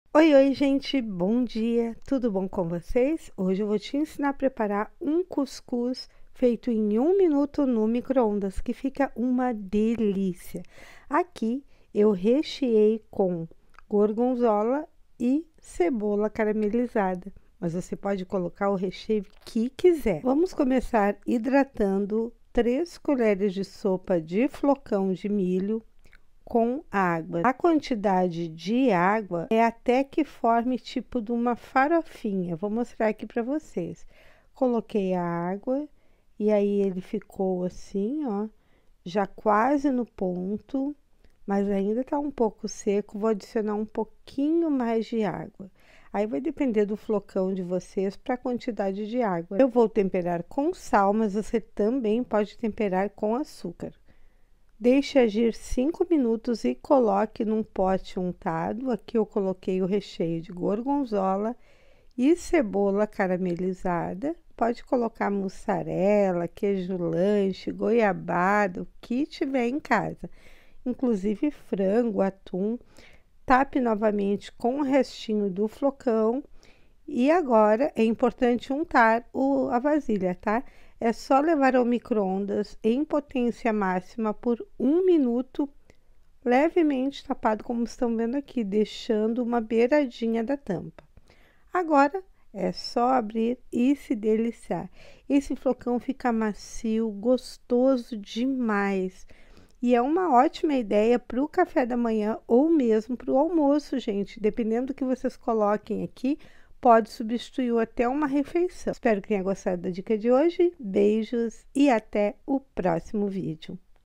Fiz em 1 minuto no Micro-ondas: Cuscuz com Gorgonzola e Cebola Caramelizada!
Olá pessoal, bom dia! Como estão? Hoje eu trago a solução para quem quer preparar uma deliciosa comida rápida e fácil: Cuscuz no micro-ondas em apenas 1 minuto! Eu recheei com  gorgonzola e cebola caramelizada, mas você pode usar outras opções. Primeiro hidratamos 3 colheres de sopa de flocão de milho com água, até que consiga se formar uma massa cremosa.